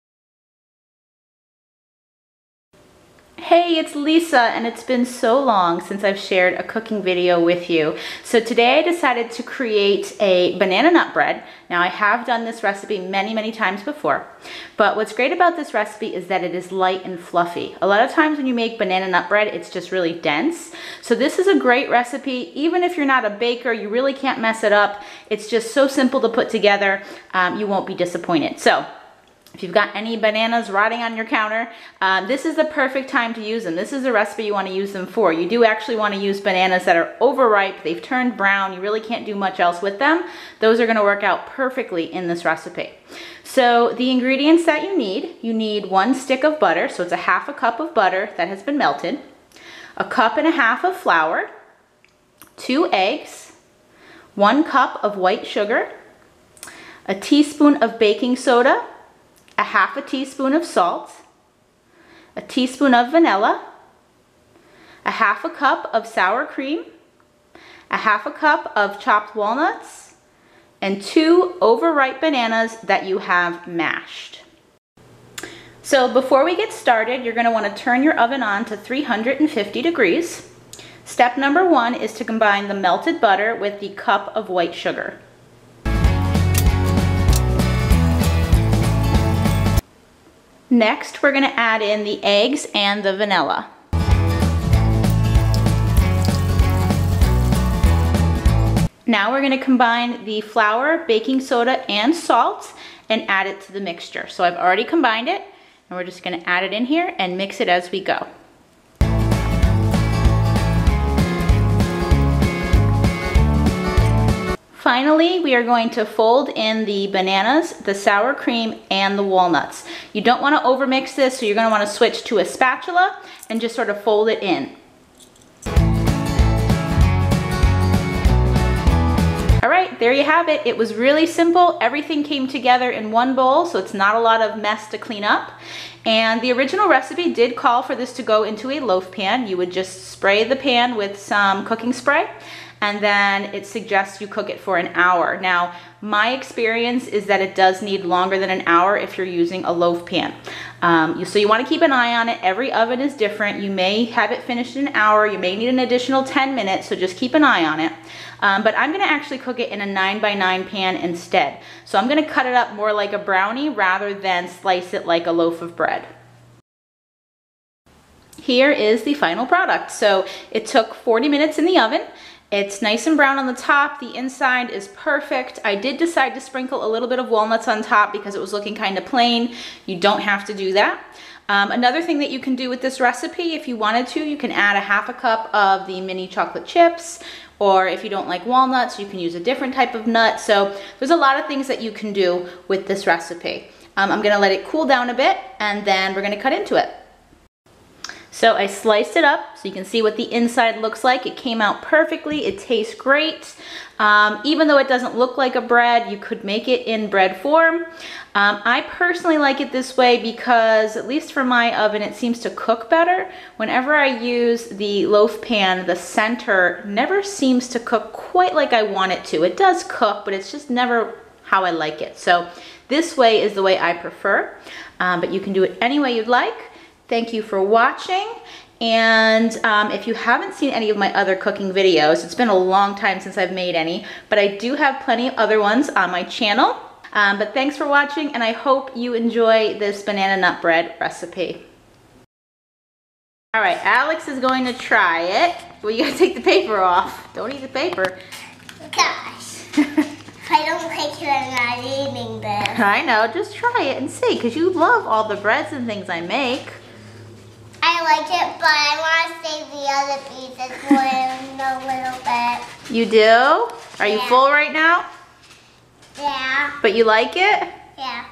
Banana Nut Bread Recipe - Light & Tasty!!
It has been a while but I've finally made a new cooking video!  This time it's a banana nut bread.  I decided to make it in a cake pan, but it could also be made in a loaf pan. I love this recipe because its not heavy like a lot of other banana bread recipes. I hope you enjoy it as much as we do!

Be sure to check my playlist of other cooking videos too! :)

Ingredients:
1/2 Cup Melted Butter
1 1/2 Cups Flour
2 Eggs
1 Cup Sugar
1 tsp Baking Soda
1/2 tsp salt
1 tsp vanilla
1/2 Cup Sour Cream
1/2 Chopped Walnuts
2 Bananas - Mashed